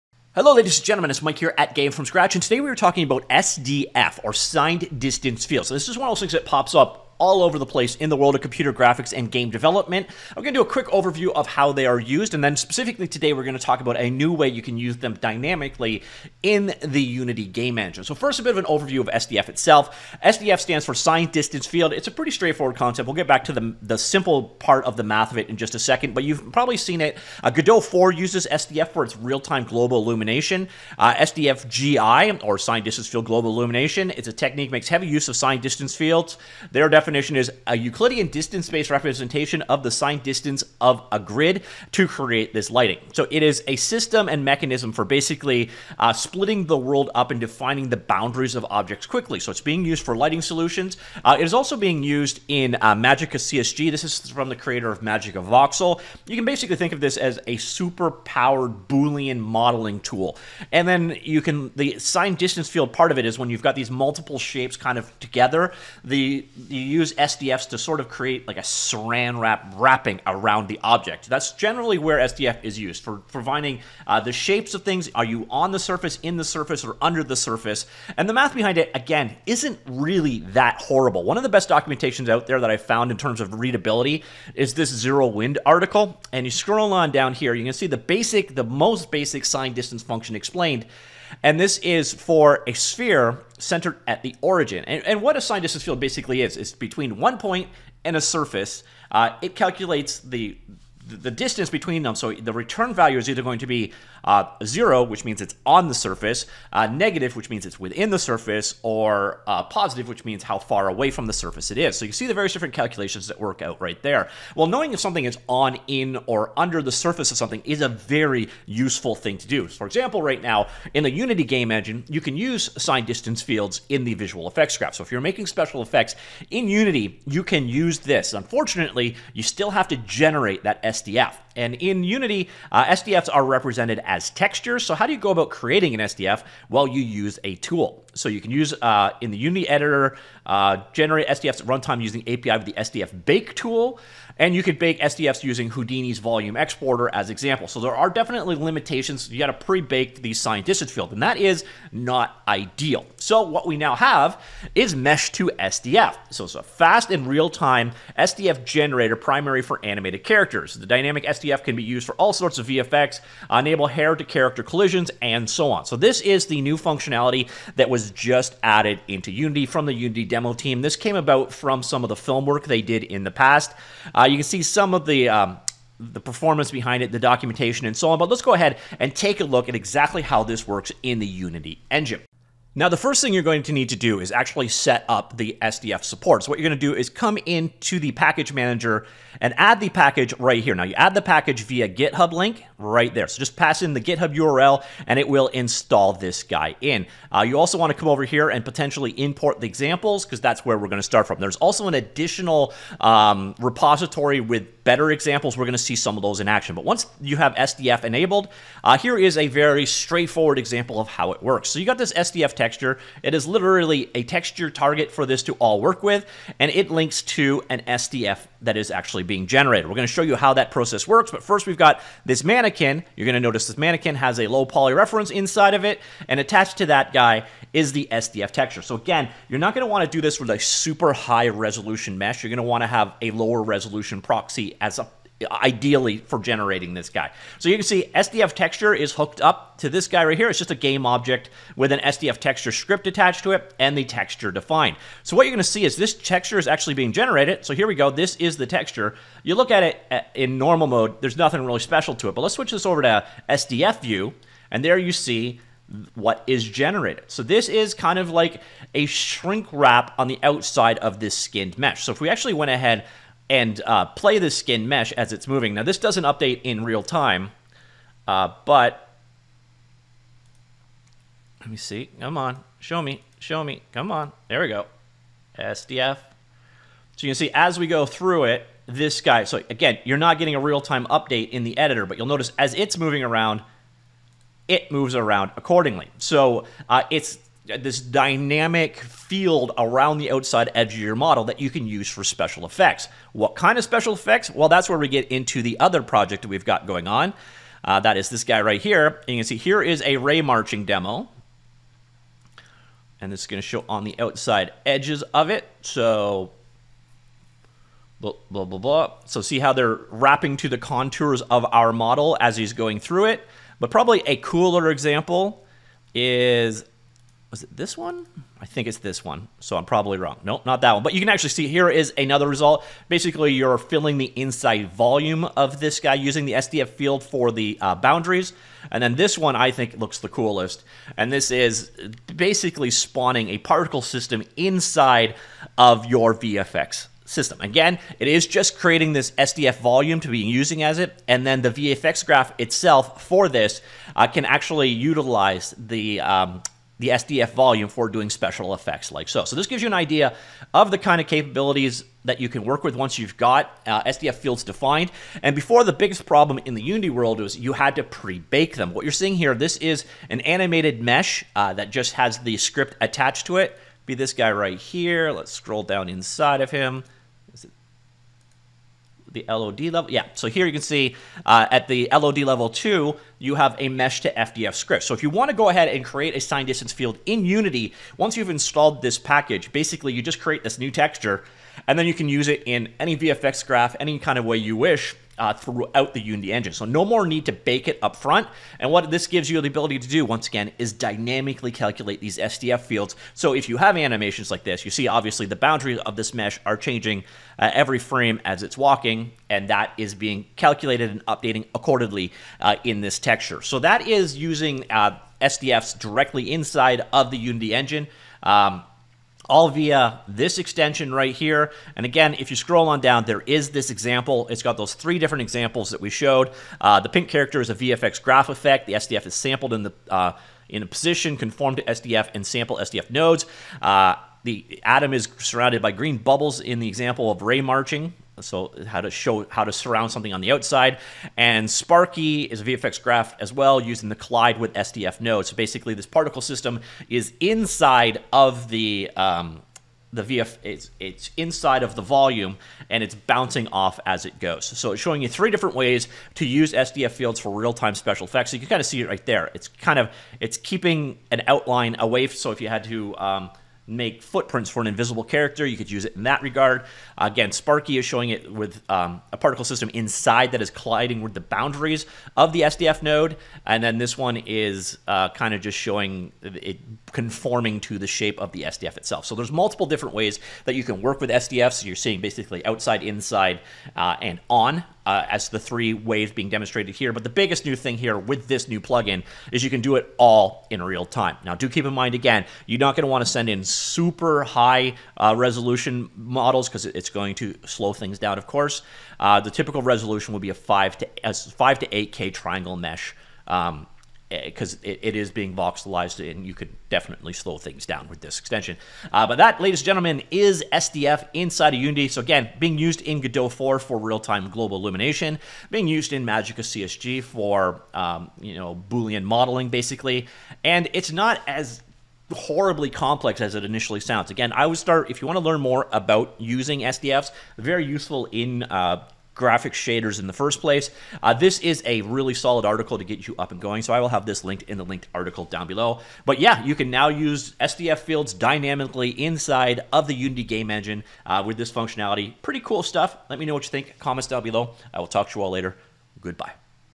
Dynamic Signed Distance Fields (SDF) -- Now In Unity -- What They Are And Why You Should Care!
SDF or Signed Distance Fields are an increasingly common site in the world of game engines and computers graphics, from the dynamic lighting in the Godot game engine, to the Boolean modeling in MagicaCSG, to thousands upon thousands of shaders, SDF are everywhere.

Everywhere now includes the ability to create dynamic SDFs in the Unity game engine, previously something that could only be pre-baked.